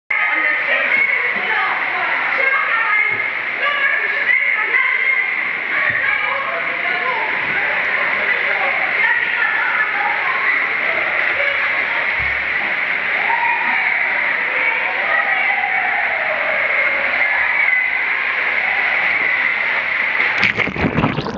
Schwimm in Gevelsberg - Kinderrutsche
Onride auf der Kinderrutsche im Freizeitbad Schwimm In Gevelsberg (Ennepe-Ruhr-Kreis). Die Rutsche ist mit der richtigen Technik sehr schnell!
Typ: Tunnelrutsche R84/600N
Länge: 38 Meter
Starthöhe: 4 Meter
Baujahr: 2000
Hersteller: Roigk
Specials: relativ schnell